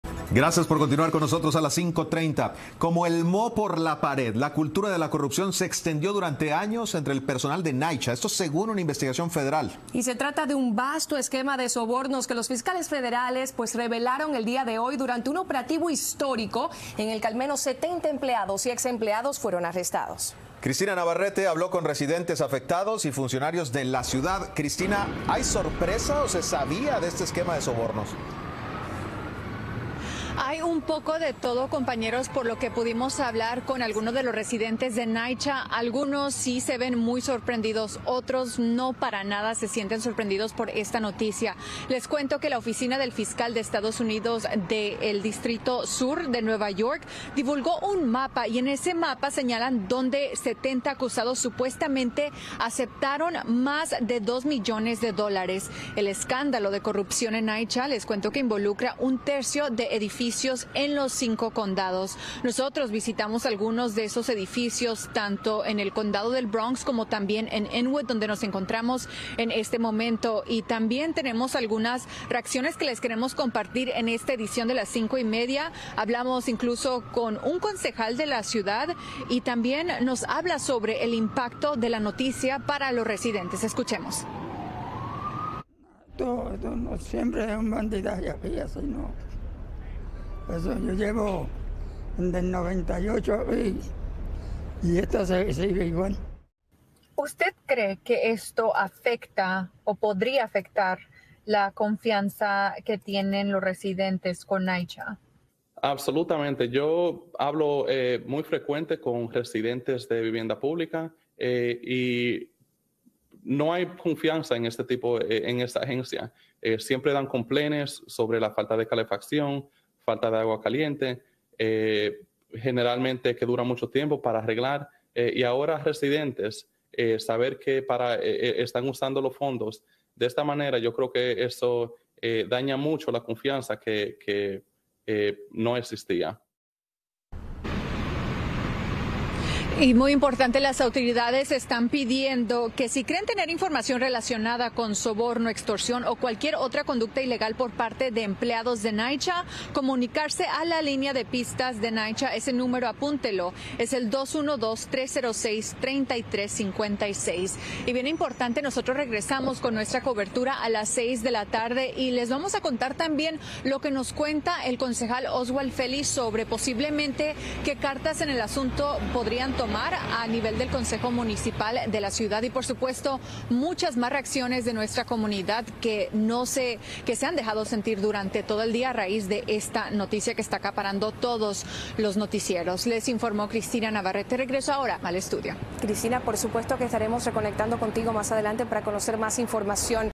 Inquilinos se expresan sobre esquema de sobornos en NYCHA
Cristina Navarrete con los detalles.